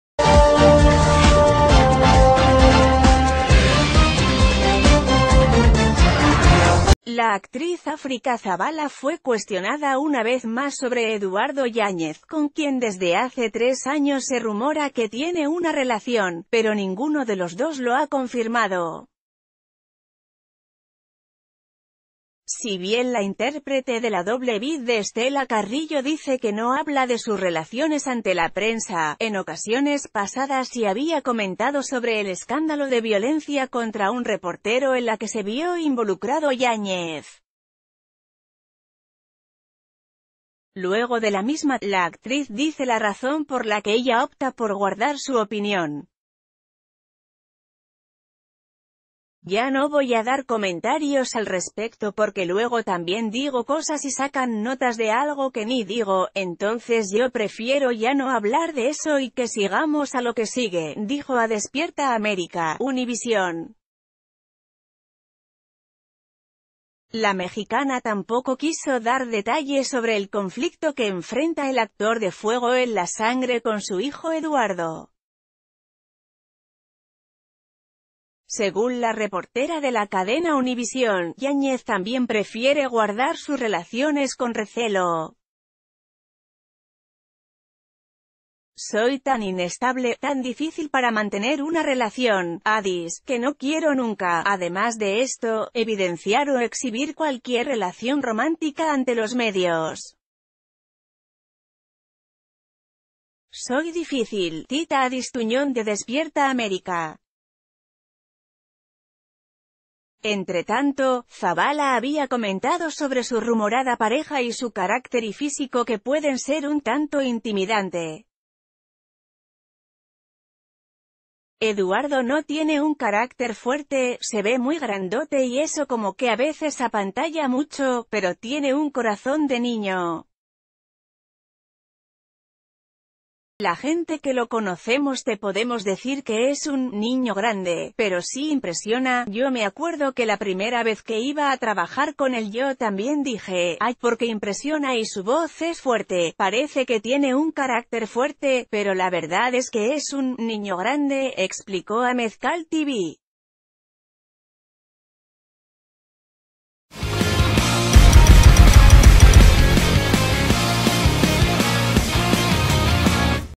África Zavala revela por qué prefiere no opinar sobre Eduardo Yáñez
La actriz África Zavala fue cuestionada una vez más sobre Eduardo Yáñez, con quien desde hace tres años se rumora que tiene una relación, pero ninguno de los dos lo ha confirmado. Si bien la intérprete de la Doble vid de Estela Carrillo dice que no habla de sus relaciones ante la prensa, en ocasione...